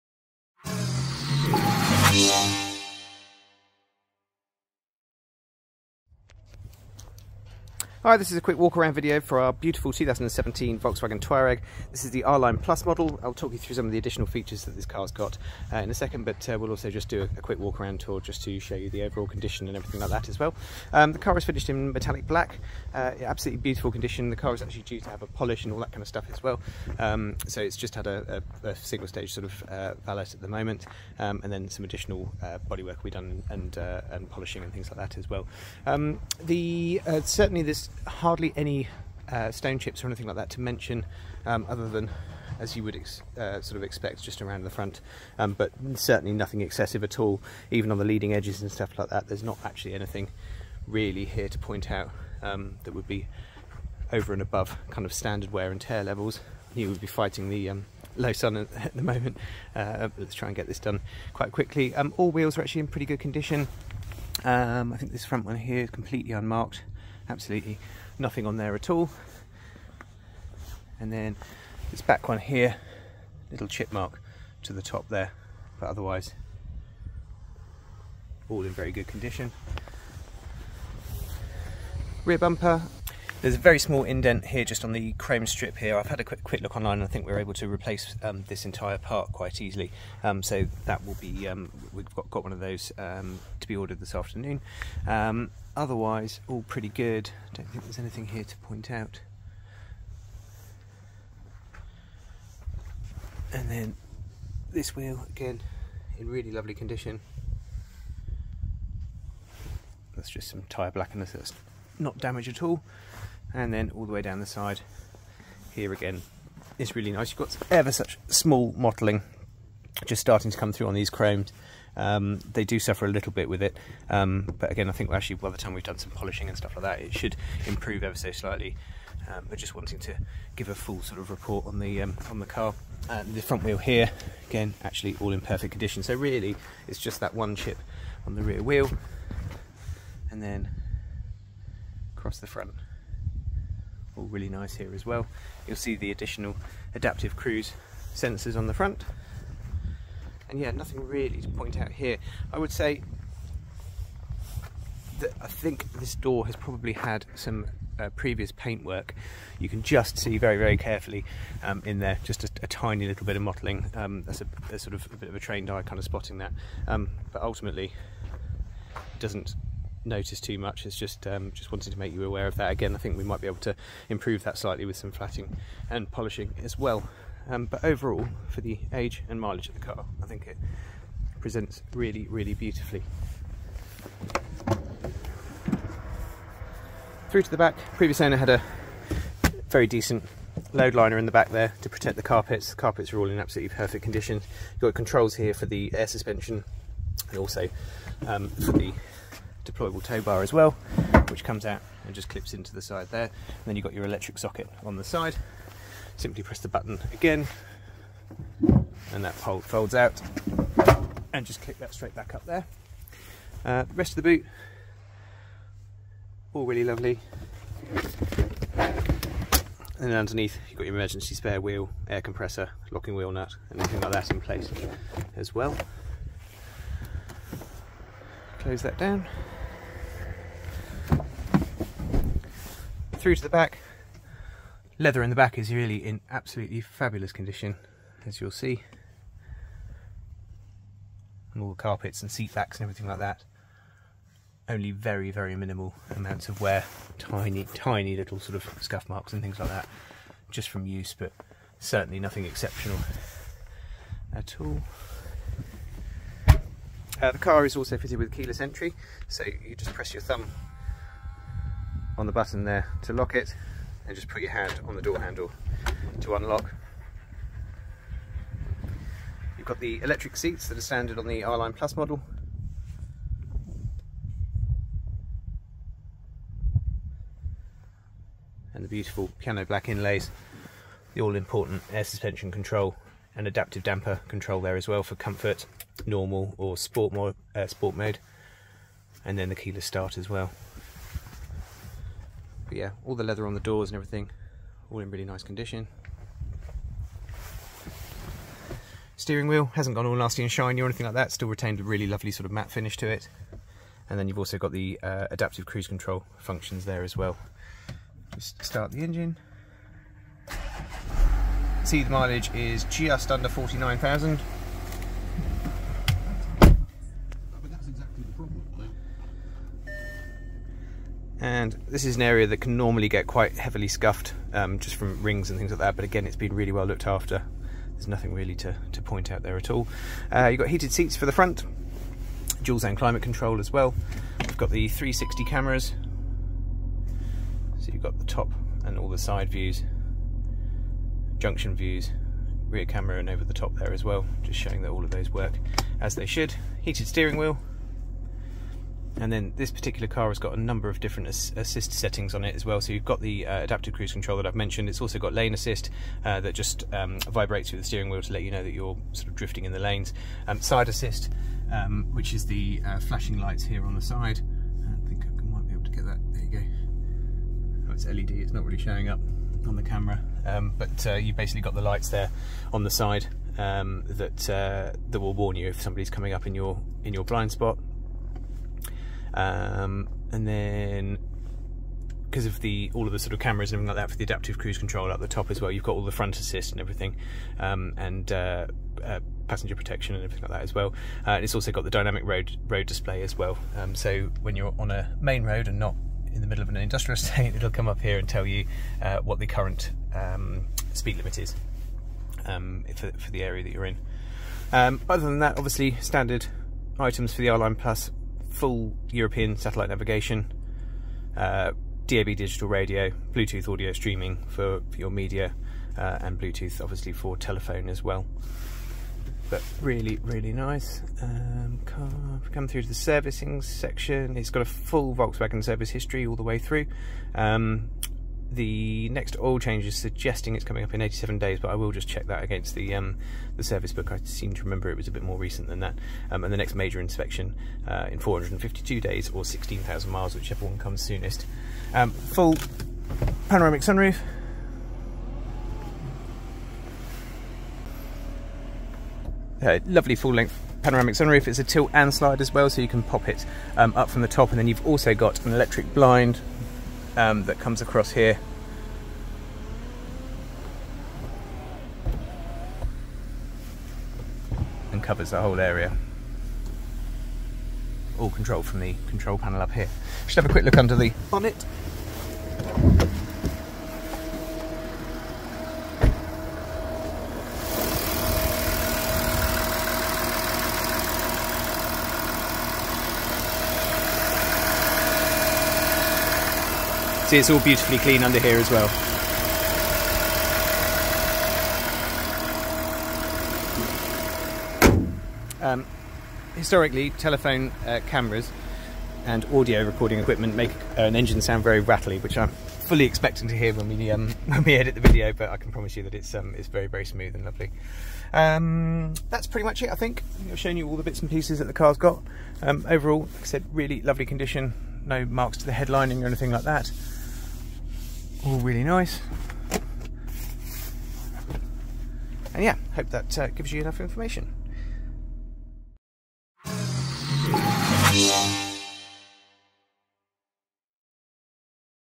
2017 VOLKSWAGEN Touareg R Line Plus with air suspension and driver assistance packages
Condition review and specification overview video for our 2017 Volkswagen Touareg R Line Plus